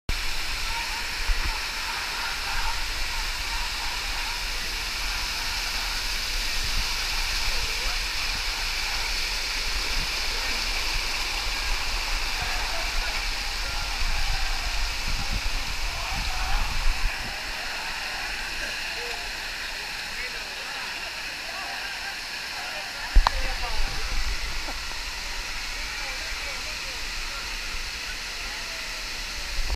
Dunns River Falls Jamaica20
Walking up Dunn's River Falls in Jamaica